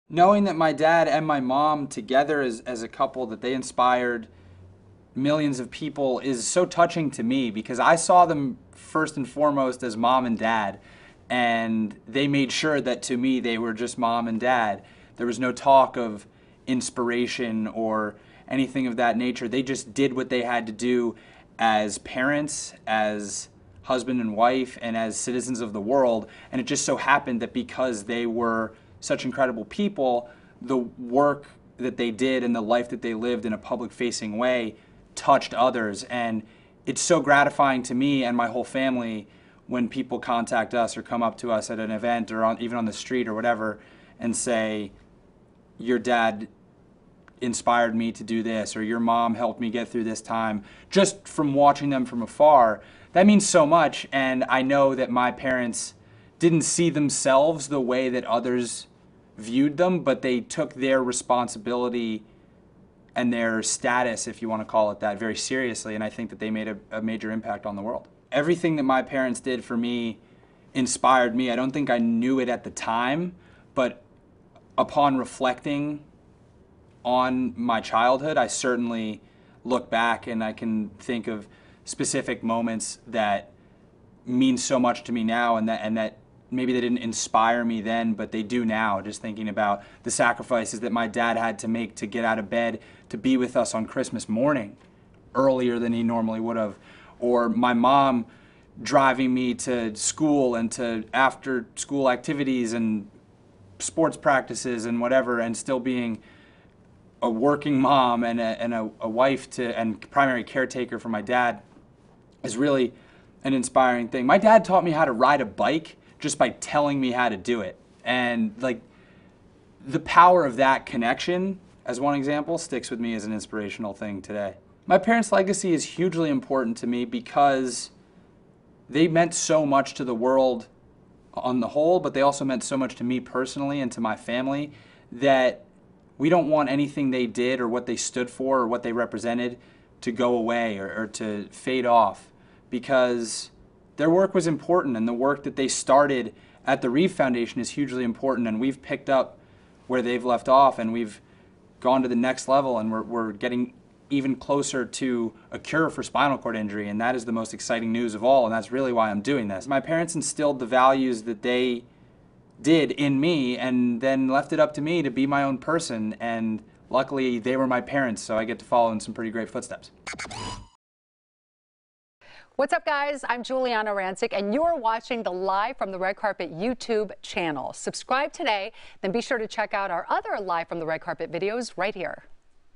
Will Reeve Talks Family's Legacy and Charity Work | E! Red Carpet & Award Shows
Christopher and Dana Reeve's son opens up on how he's working to keep his parents' legacy and charity work alive through the Reeve Foundation.